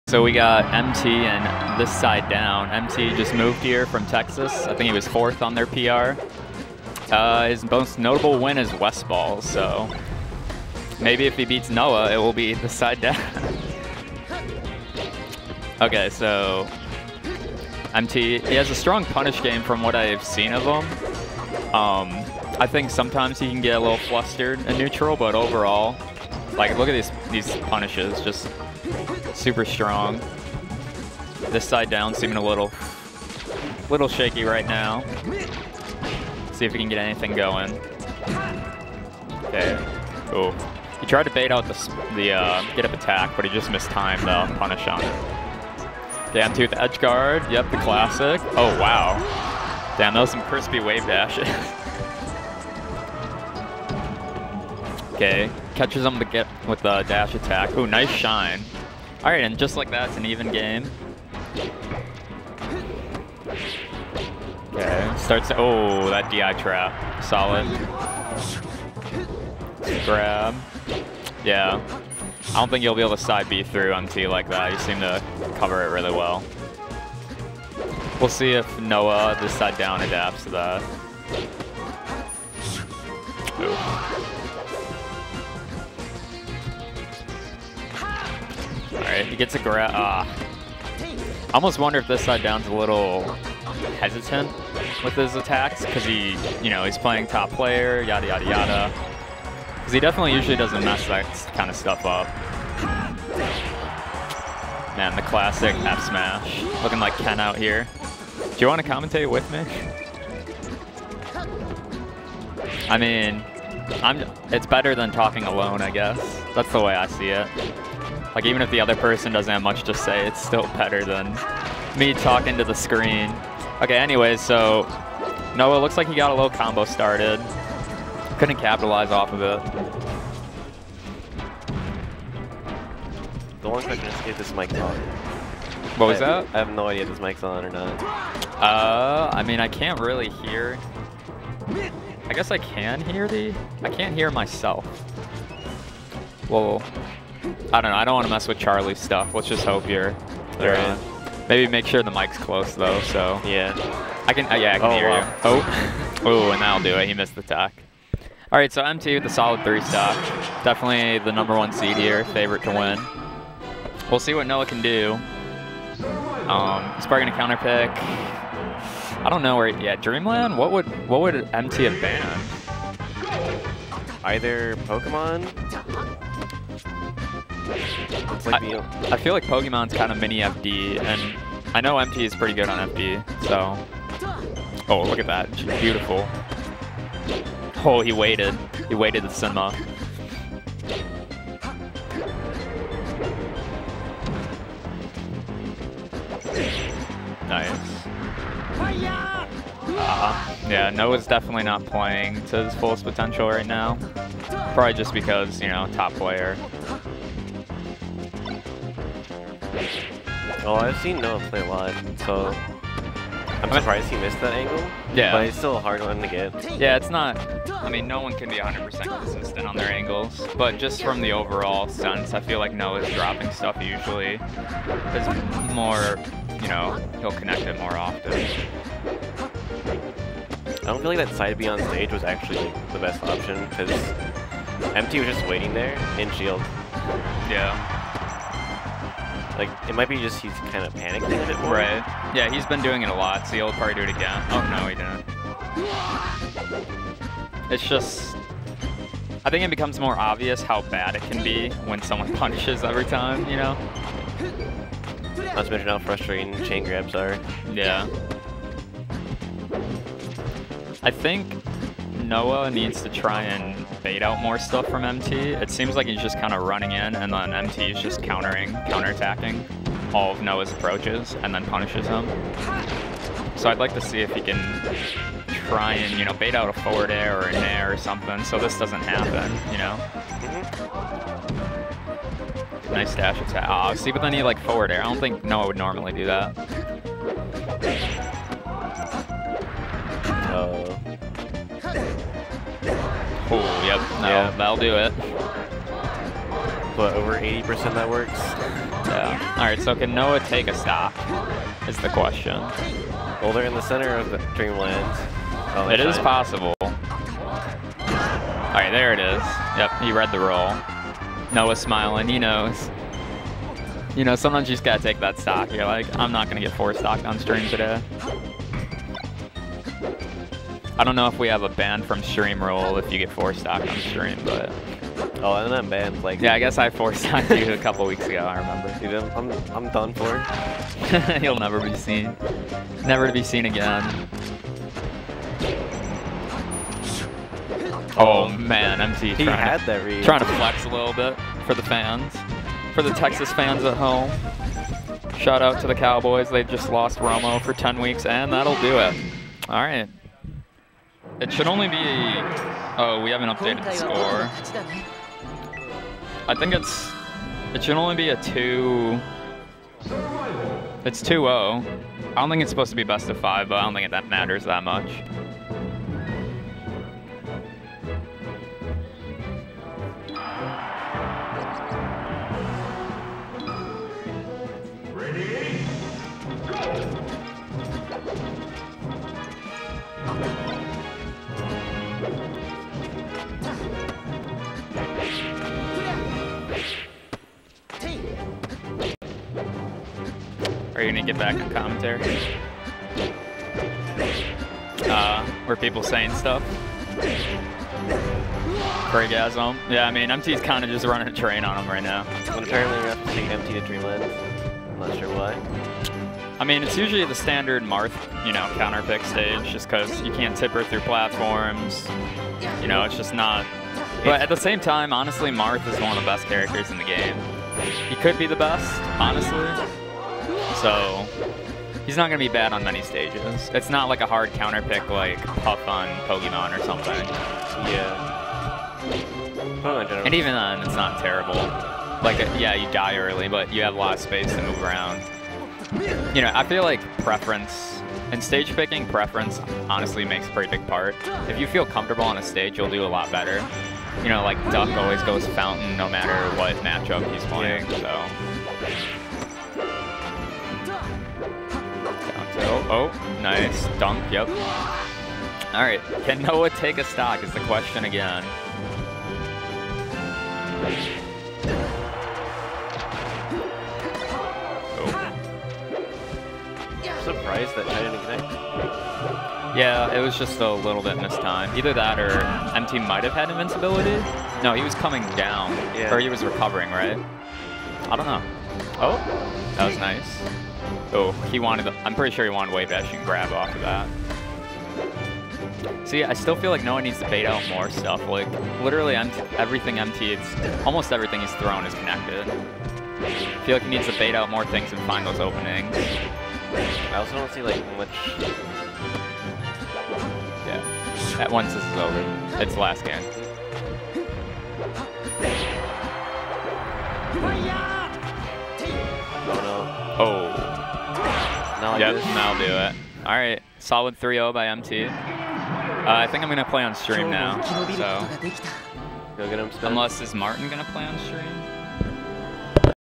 MTSD9 - MT Vs ThisSideDown - Winners Semifinals
Madtown Smackdown 9 | Sunday, August 28th, 2016 | Madison, WI